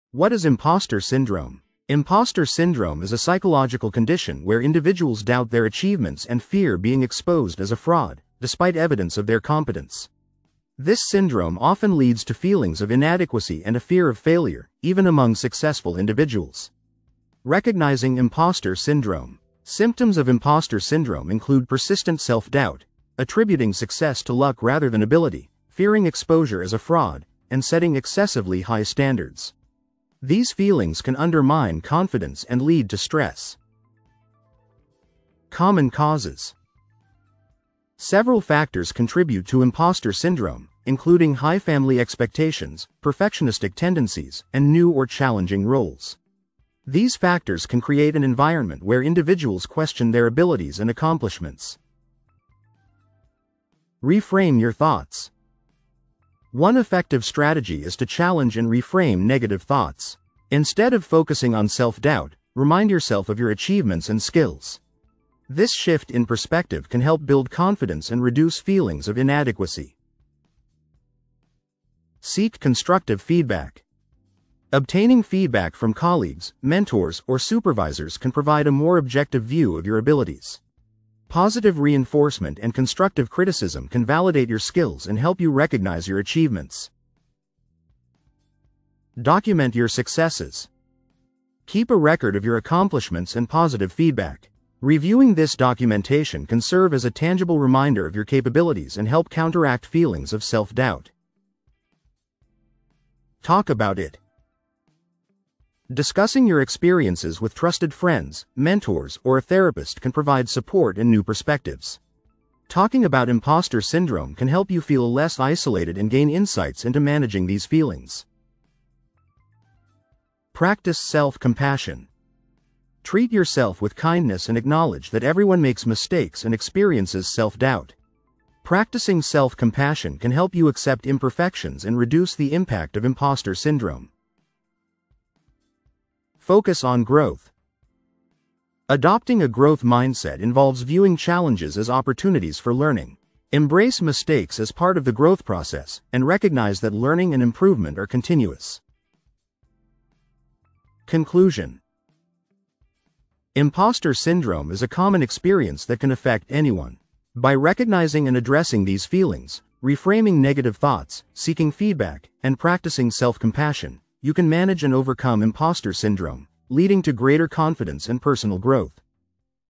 What is Imposter Syndrome, and How Can You Overcome It
Imposter Syndrome affects many people. Learn what it is and how to overcome self-doubt and achieve your true potential.